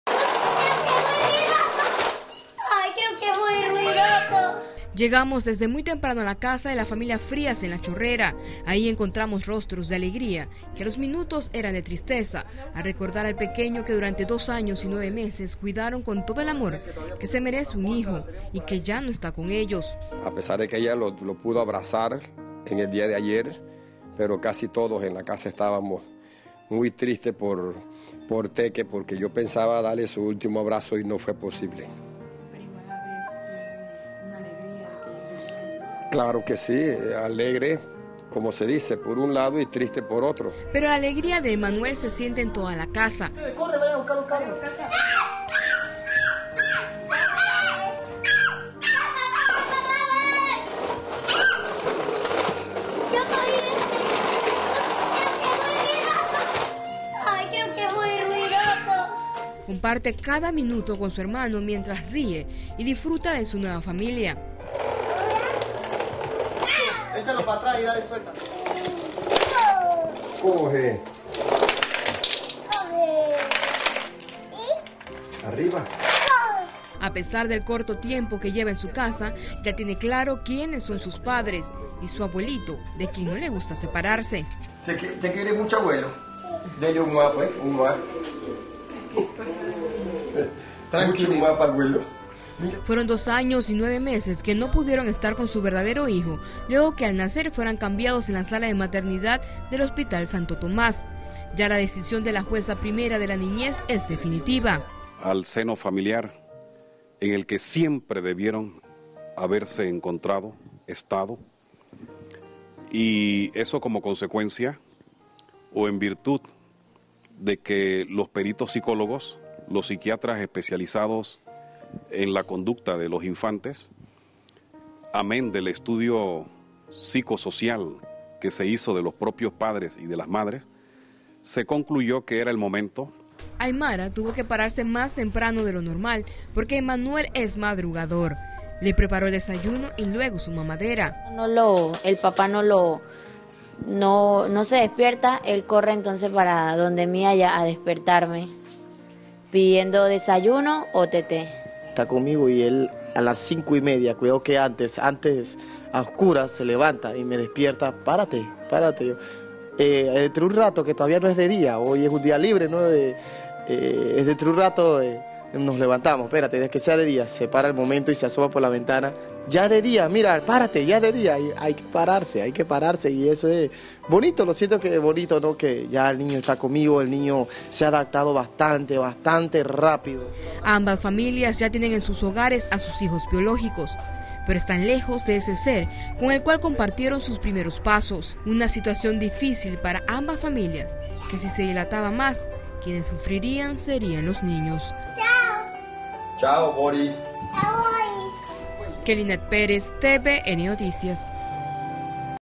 Caso de niños cambiados en Panamá - Final agridulce | TVN Panamá
El final del caso de los niños cambiados en la Maternidad del Hospital Santo Tomás tiene un sabor agridulce. La familia Frías, por orden de la Jueza Primera de la Niñez, tiene a su hijo biológico y la otra familia tiene al suyo, pero al mismo tiempo queda la tristeza de no poder tener al otro niños, con el que compartieron por más de dos años. Más en tvn-2.com